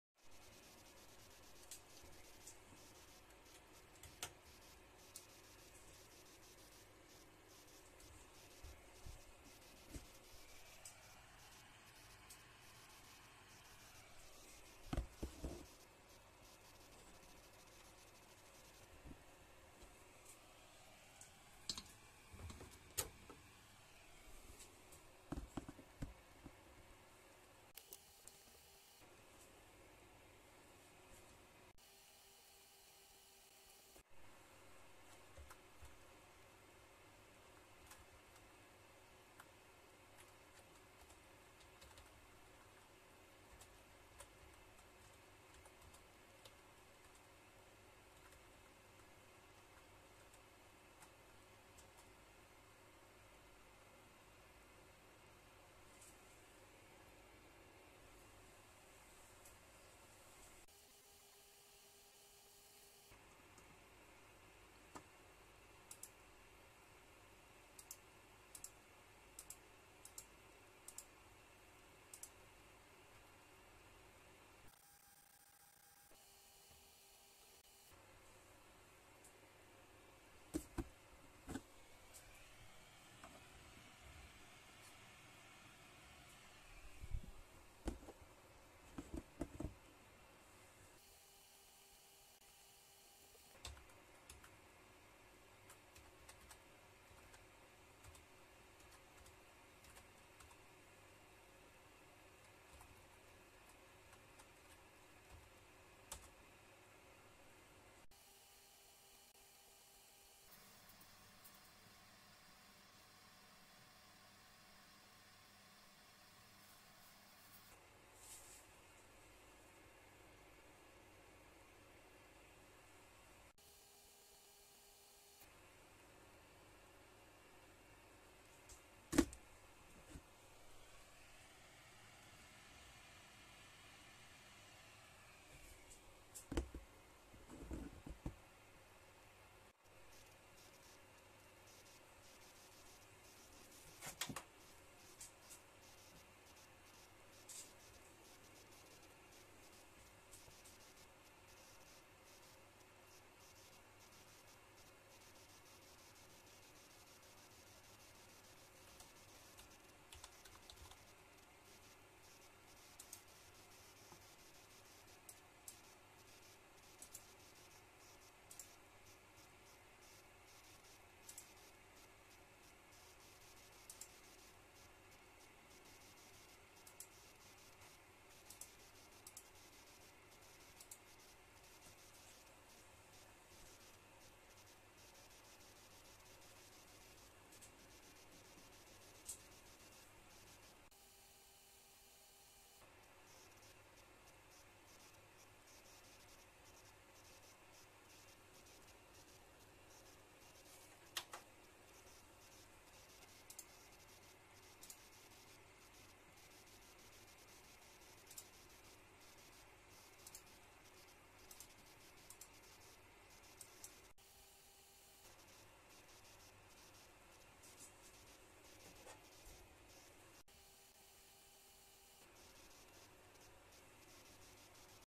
Asus Rog Strix B550-I Gaming - ErP Bug + High Power Consumption in Standby
After flashing the BIOS I enabled the Stealth Mode, waited for Standby and the Board consumed 3.6Watts. Then I enabled ErP and waited again for Standby. This time the board even consumes 5.3W. Shutting down windows with Fastboot disabled is a workaround, but does not solve the high power usage in Standby.